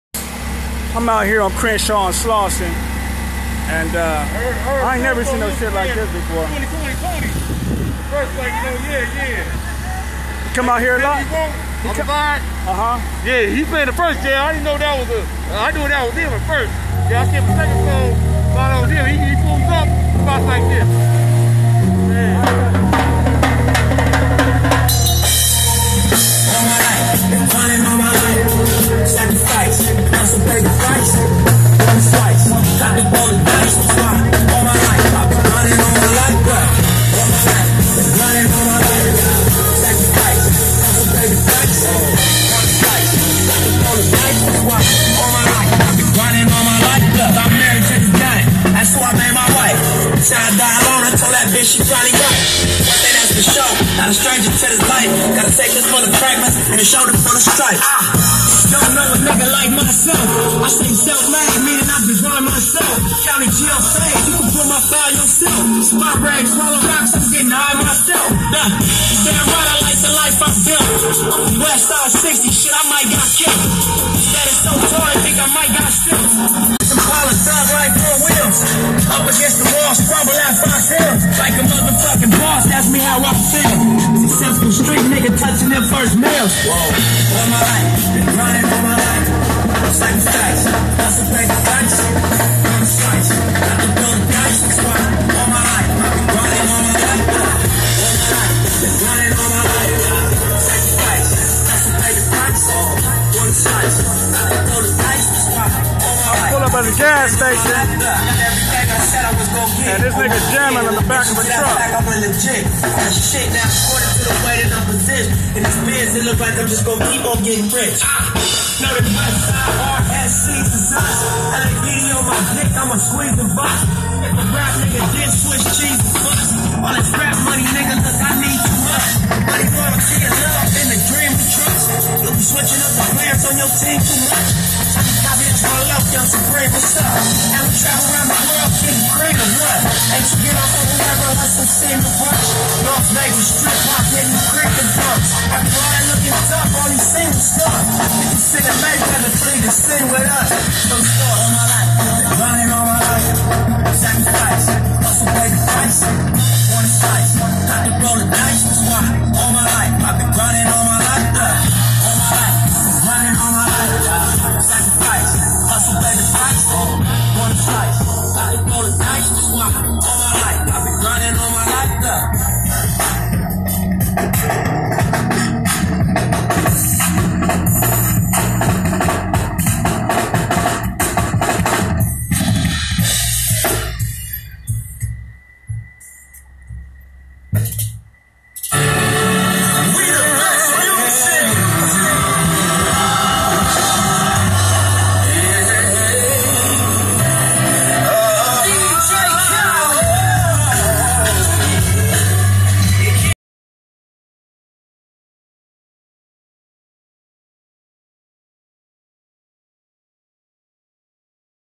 drummer on crenshaw and slauson in the back of a truck
drummer in back of truck live